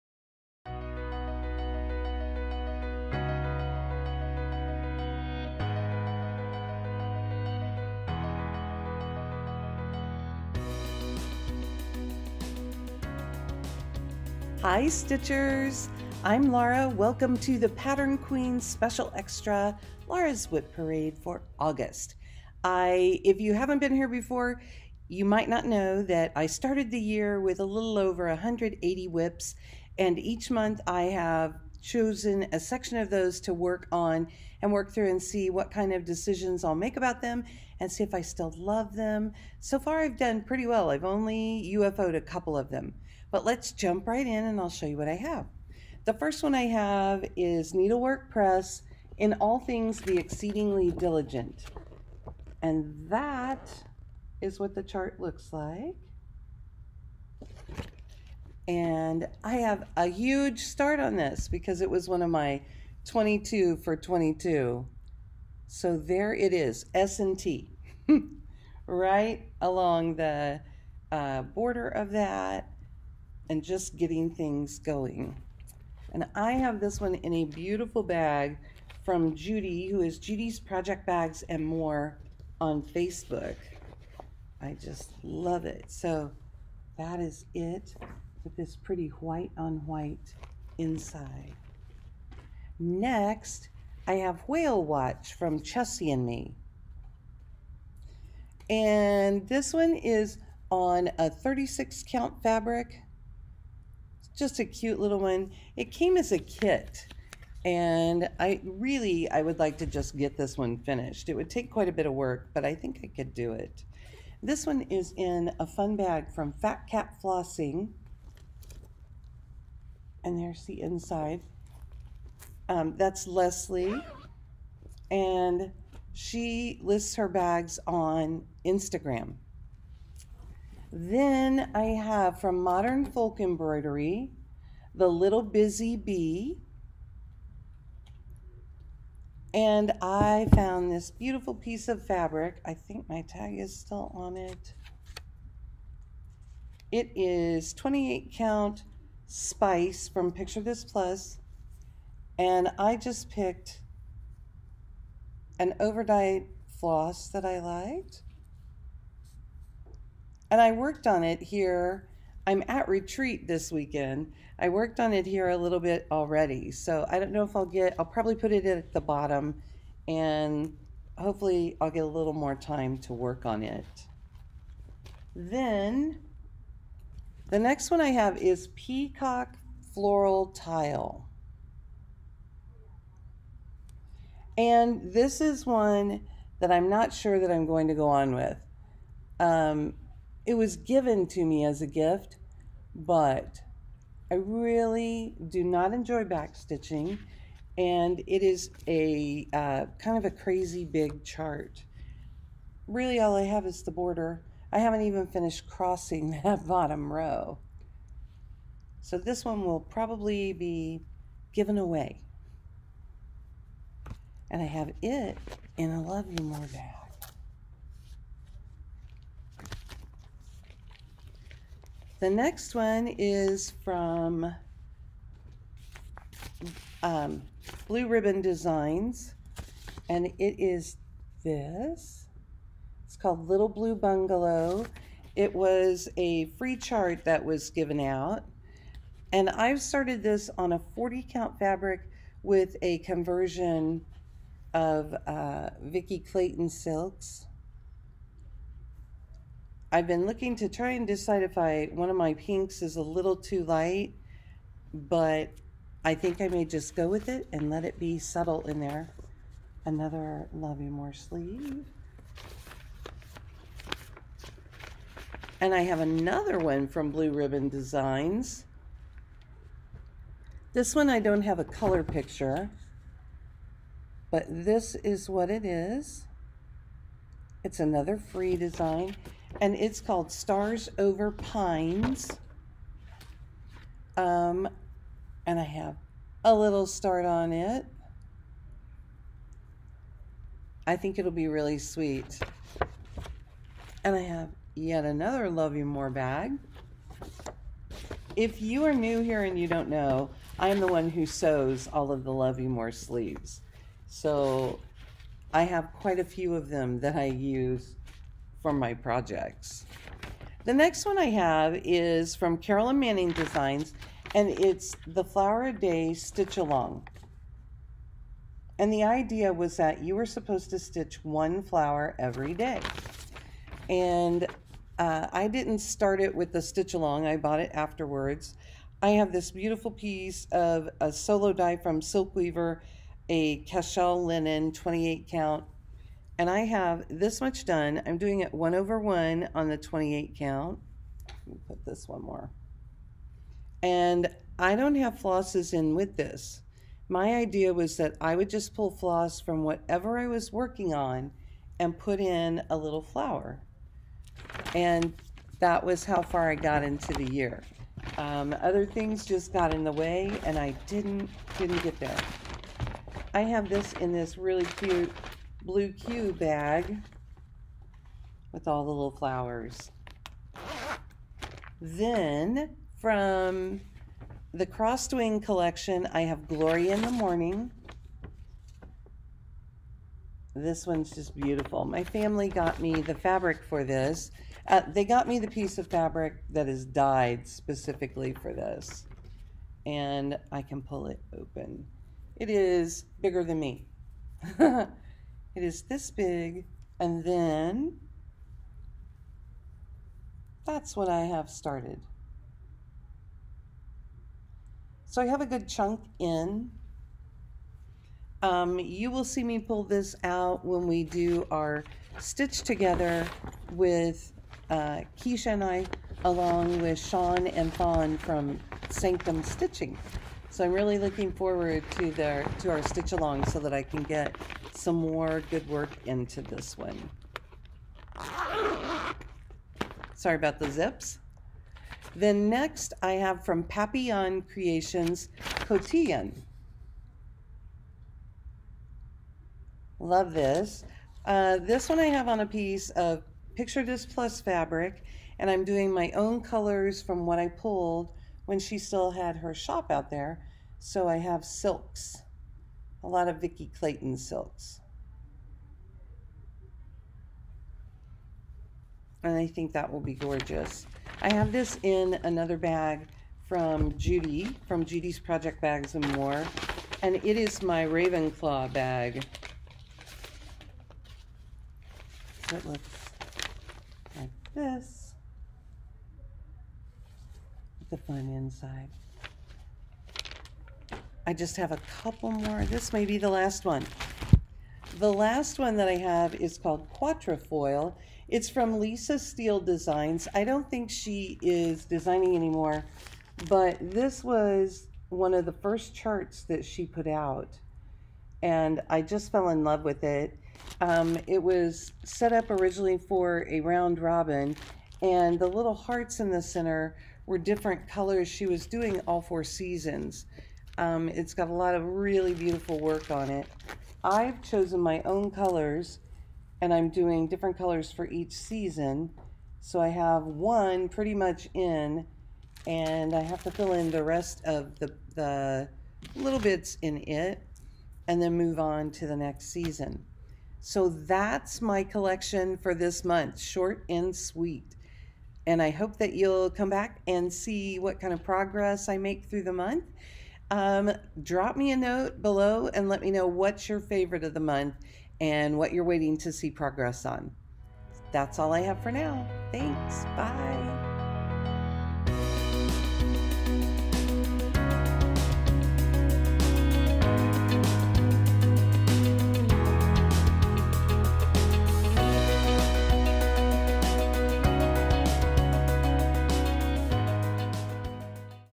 The Pattern Queens Special Extra: Laura's August WIP Parade
I show the WIPs I'll be working on during the month. You'll see them all again as I stitch on them throughout the month. Let me know which projects you like best!